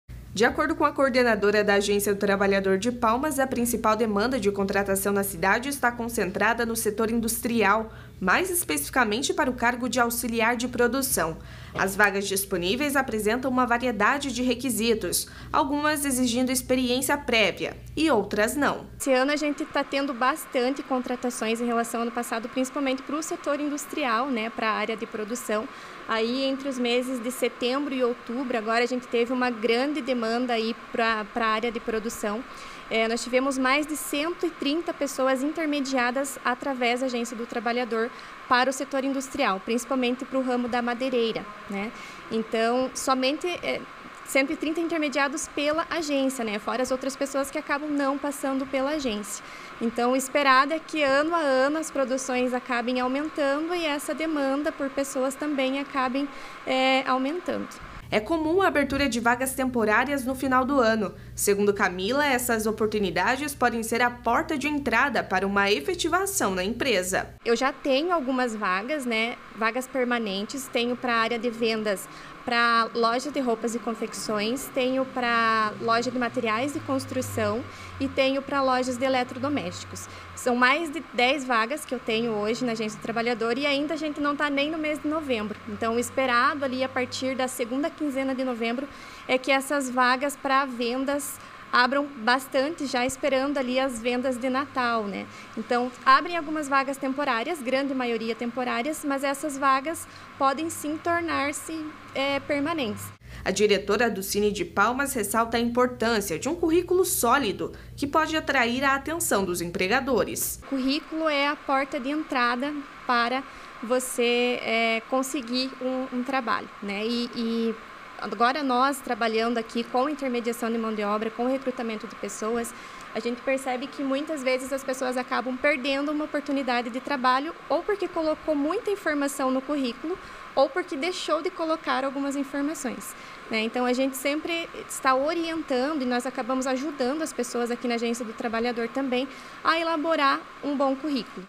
Em Palmas o número de contratações para o mercado de trabalho está em alta
Cidadania: Especialmente com a chegada do final de ano com a abertura de vagas temporárias.